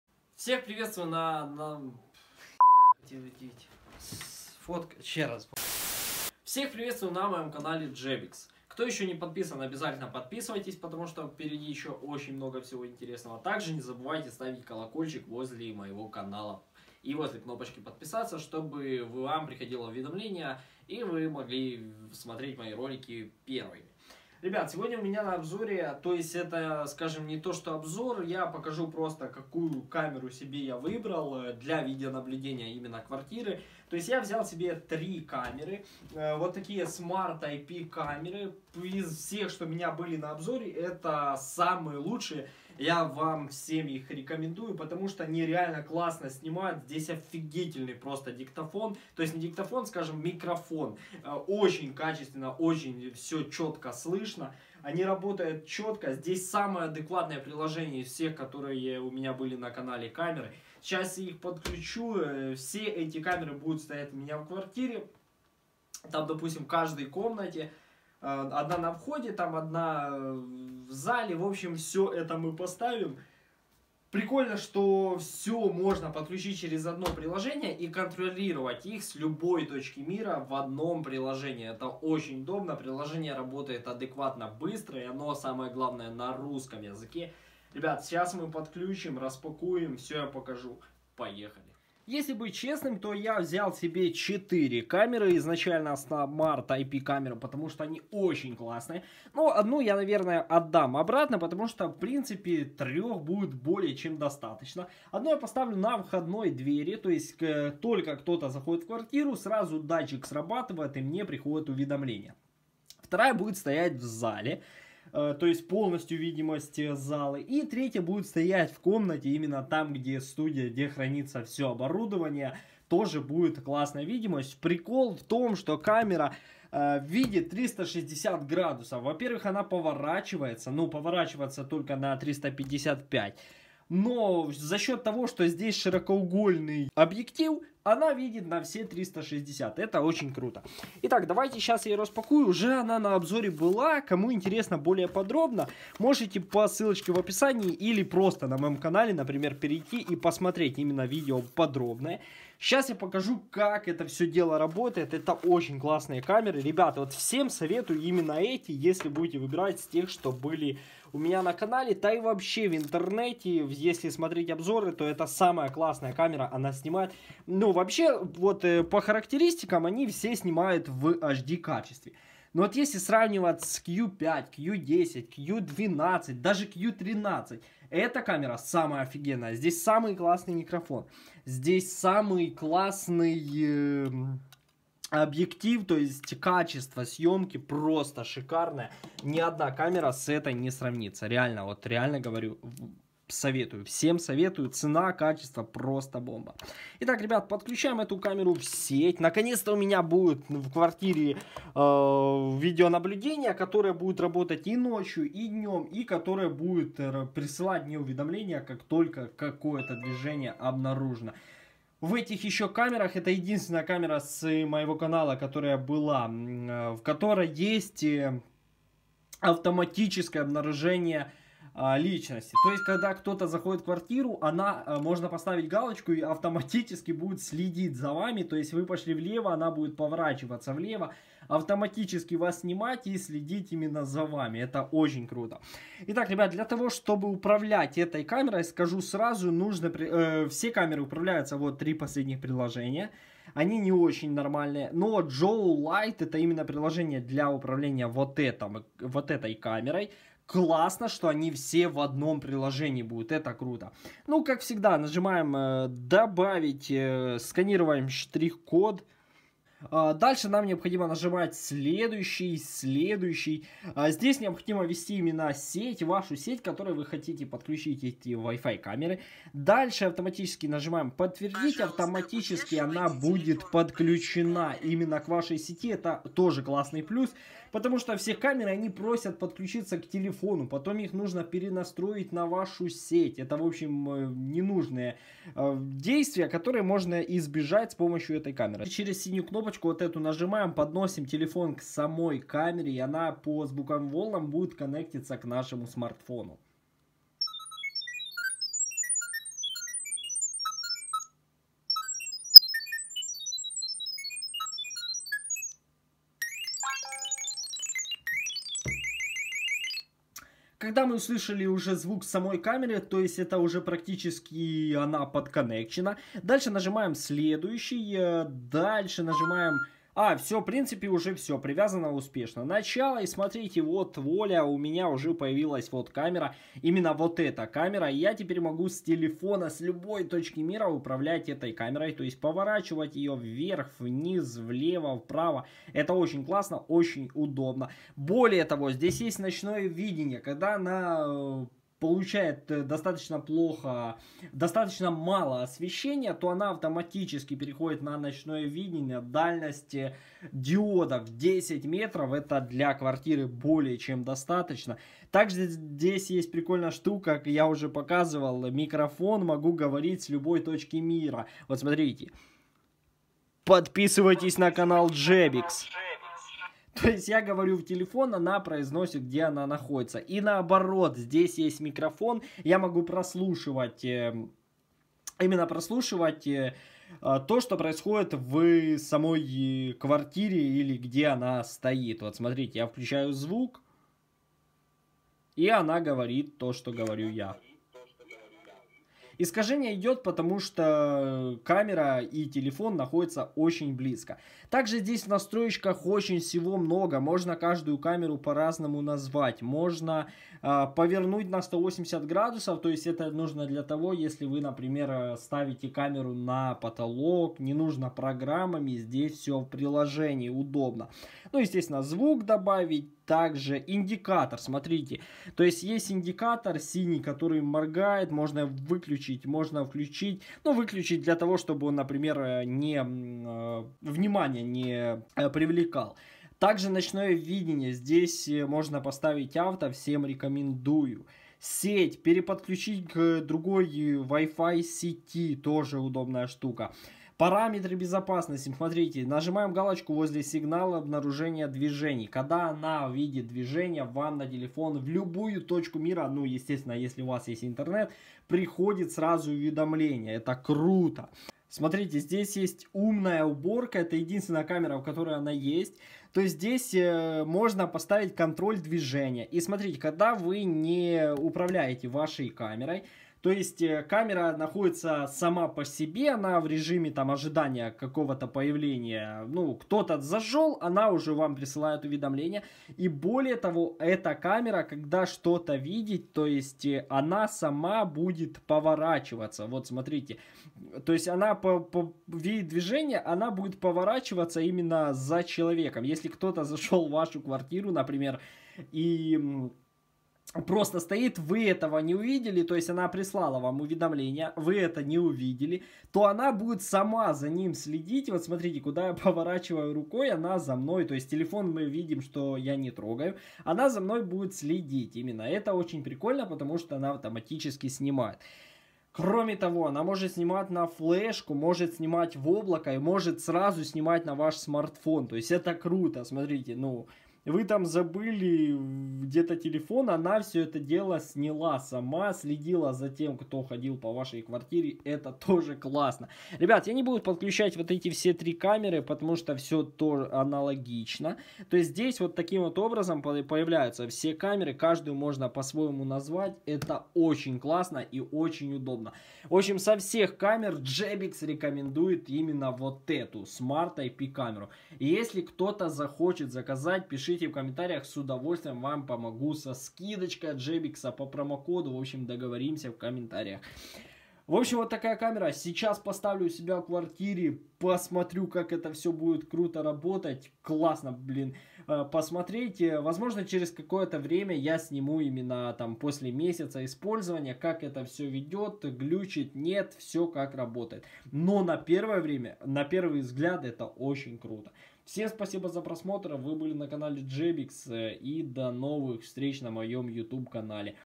Обзор вайфай Камеры Видеонаблюдения Датчики движения для моей Квартиры
Обзор моей новой охранной системы ! Камеры видеонаблюдения с датчиками движения ! Управления с любой точки мира через телефон или пк ! Ночная и дневная автоматическая сЬемка ! И Это все По адекватной цене ! ВСЕМ РЕКОМЕНДУЮ ТАКИЕ КАМЕРЫ !